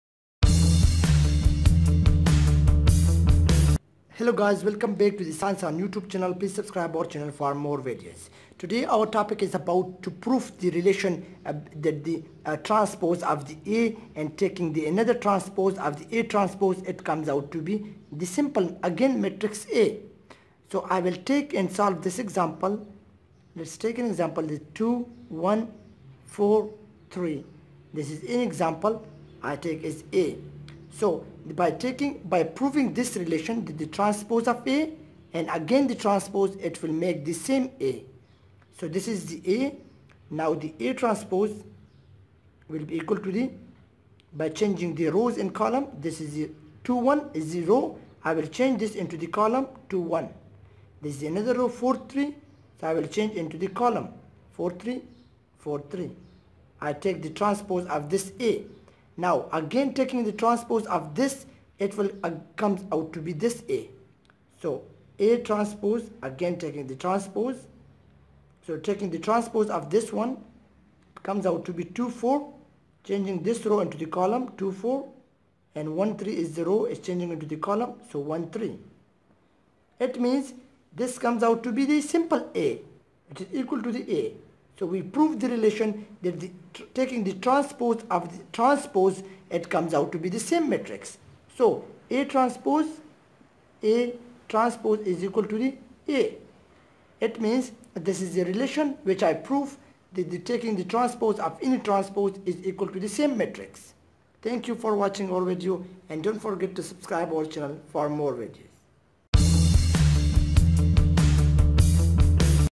Transpose of a transpose proof || Math is Easy
This video shows the transpose of a transpose. Transpose is basically changing the row of a matrix to column.

transpose
matrix transpose
row matrix
column matrix